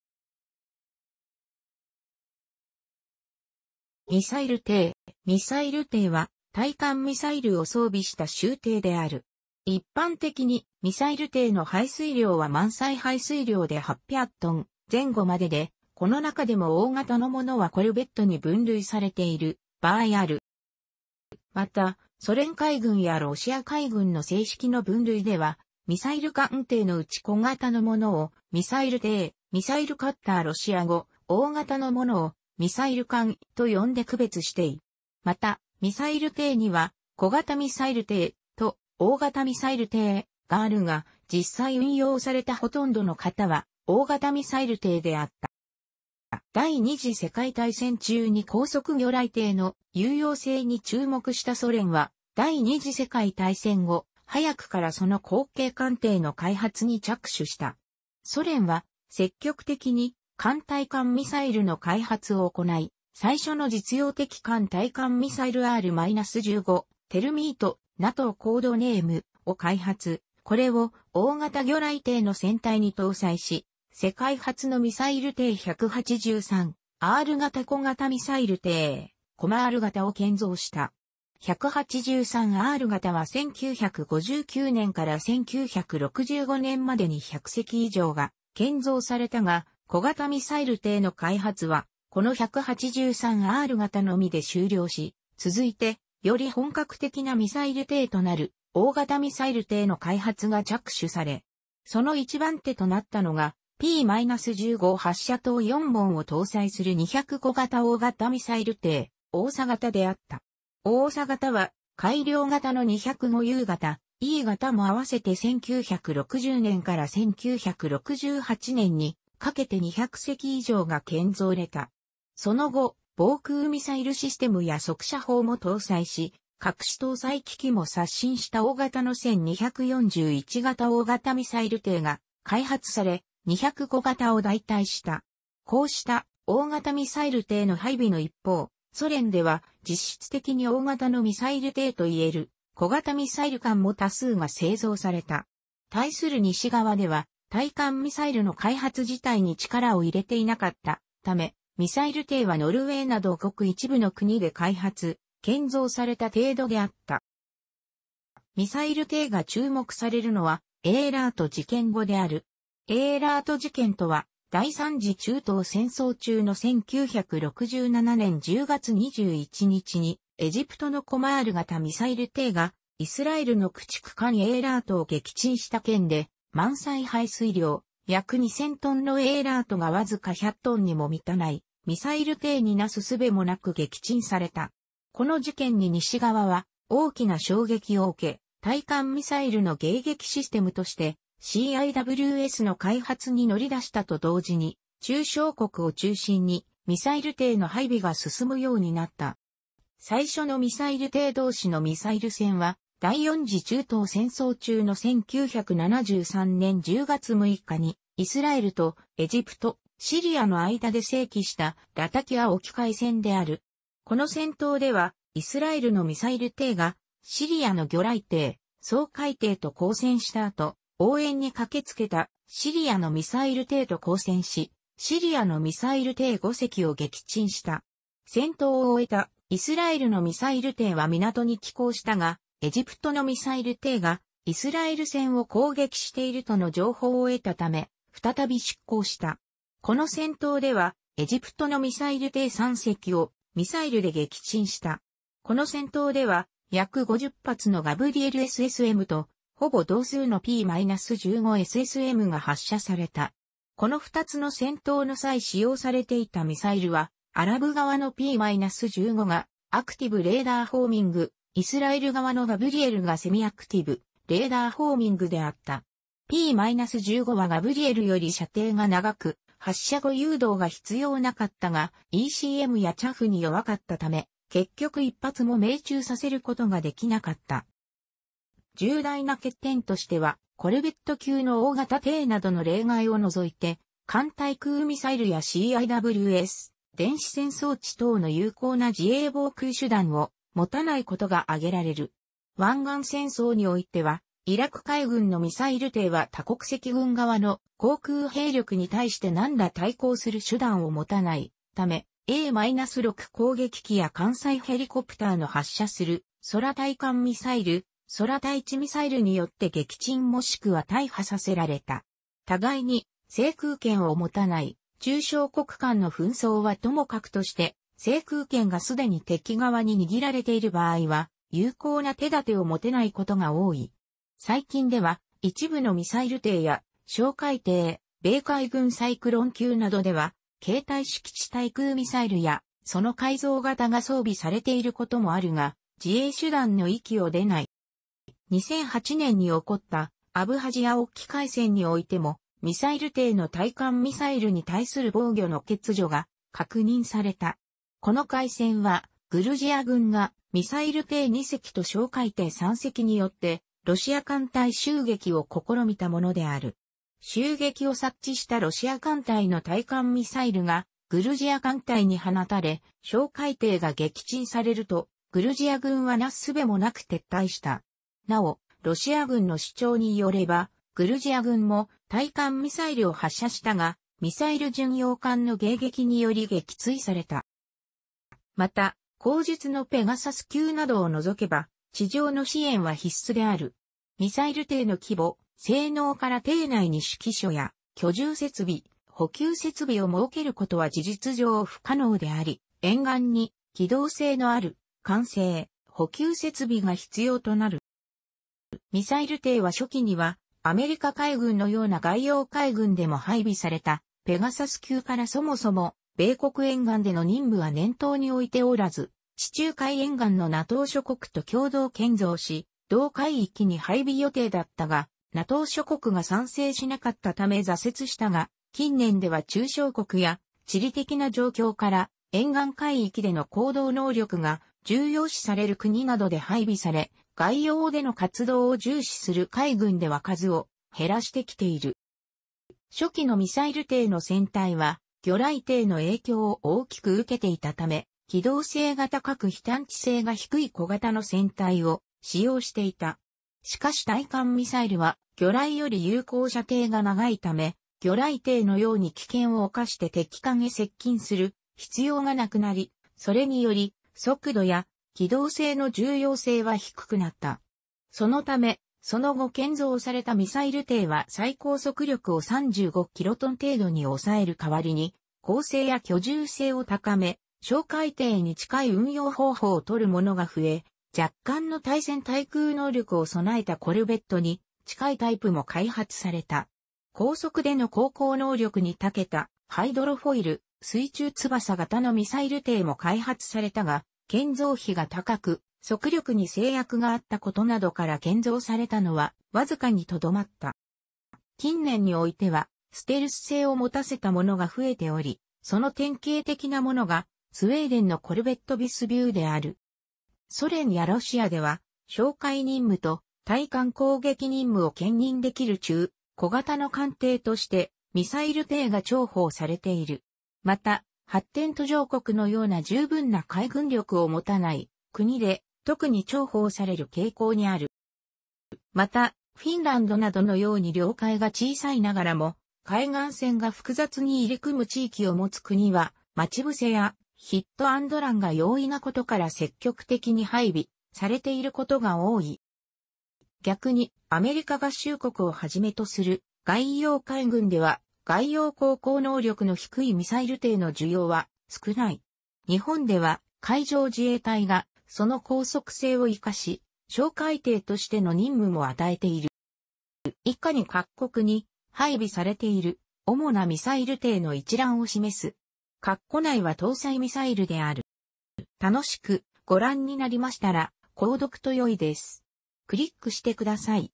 ミサイル艇
ミサイル艇（ミサイルてい）は、対艦ミサイルを装備した舟艇である。一般的にミサイル艇の排水量は満載排水量で800トン前後までで、この中でも大型のものはコルベットに分類されている場合もある。

また、ソ連海軍やロシア海軍の正式の分類では、ミサイル艦艇のうち小型のものを「ミサイル艇」（「ミサイル・カッター」；ロシア語:'）、大型のものを「ミサイル艦」（'）と呼んで区別している。また、「ミサイル艇」には「小型ミサイル艇」（'）と「大型ミサイル艇」（'）があるが、実際運用されたほとんどの型は「大型ミサイル艇」であった。

第二次世界大戦中に高速魚雷艇の有用性に注目したソ連は、第二次世界大戦後、早くからその後継艦艇の開発に着手した。ソ連は積極的に艦対艦ミサイルの開発を行い、最初の実用的艦対艦ミサイルR-15「テルミート」（NATOコードネーム:）を開発、これを「大型魚雷艇」の船体に搭載し世界初のミサイル艇183R型小型ミサイル艇（コマール型）を建造した。183R型は1959年から1965年までに100隻以上が建造されたが「小型ミサイル艇」の開発はこの183R型のみで終了し、続いてより本格的なミサイル艇となる「大型ミサイル艇」の開発が着手された。その一番手となったのが、P-15発射筒4門を搭載する205型大型ミサイル艇（オーサ型）であった。オーサ型は改良型の205U型（II型）も合わせて1960年から1968年にかけて200隻以上が建造された。その後、防空ミサイルシステムや速射砲も搭載し各種搭載機器も刷新した大型の1241型大型ミサイル艇が開発され、205型を代替した。こうした「大型ミサイル艇」の配備の一方、ソ連では実質的に大型のミサイル艇といえる「小型ミサイル艦」も多数が製造された。対する西側では対艦ミサイルの開発自体に力を入れていなかったため、ミサイル艇はノルウェーなどごく一部の国で開発、建造された程度であった。

ミサイル艇が注目されるのはエイラート事件後である。エイラート事件とは、第三次中東戦争中の1967年10月21日にエジプトのコマール型ミサイル艇がイスラエルの駆逐艦エイラートを撃沈した事件で、満載排水量約2,000トンのエイラートがわずか100トンにも満たないミサイル艇になすすべもなく撃沈された。この事件に西側は大きな衝撃を受け、対艦ミサイルの迎撃システムとしてCIWSの開発に乗り出したと同時に、中小国を中心にミサイル艇の配備が進むようになった。

最初のミサイル艇同士のミサイル戦は第四次中東戦争中の1973年10月6日にイスラエルとエジプト、シリアの間で生起したラタキア沖海戦である。この戦闘ではイスラエルのミサイル艇がシリアの魚雷艇、掃海艇と交戦した後、応援に駆けつけたシリアのミサイル艇と交戦し、シリアのミサイル艇5隻を撃沈した。戦闘を終えたイスラエルのミサイル艇は港に寄港したが、エジプトのミサイル艇がイスラエル船を攻撃しているとの情報を得たため、再び出航した。この戦闘ではエジプトのミサイル艇3隻をミサイルで撃沈した。この戦闘では約50発のガブリエルSSMとほぼ同数のP-15 SSMが発射された。この2つの戦闘の際使用されていたミサイルはアラブ側のP-15がアクティブ・レーダー・ホーミング、イスラエル側のガブリエルがセミアクティブ・レーダー・ホーミングであった。P-15はガブリエルより射程が長く、発射後誘導が必要なかったがECMやチャフに弱かったため、結局1発も命中させることができなかった。

重大な欠点としては、コルベット級の大型艇などの例外を除いて、艦対空ミサイルやCIWS、電子戦装置等の有効な自衛防空手段を持たない事があげられる。湾岸戦争においては、イラク海軍のミサイル艇は多国籍軍側の航空兵力に対して何ら対抗する手段を持たないため、A-6攻撃機や艦載ヘリコプターの発射する空対艦ミサイル、空対地ミサイルによって撃沈もしくは大破させられた。互いに制空権を持たない中小国間の紛争はともかくとして、制空権がすでに敵側に握られている場合は有効な手立てを持てない事が多い。最近では、一部のミサイル艇や哨戒艇（米海軍サイクロン級など）では携帯式地対空ミサイルやその改造型が装備されている事もあるが、自衛手段の域を出ない。

2008年に起こったアブハジア沖海戦においても、ミサイル艇の対艦ミサイルに対する防御の欠如が確認された。この海戦はグルジア軍がミサイル艇2隻と哨戒艇3隻によって、ロシア艦隊襲撃を試みたものである。襲撃を察知したロシア艦隊の対艦ミ...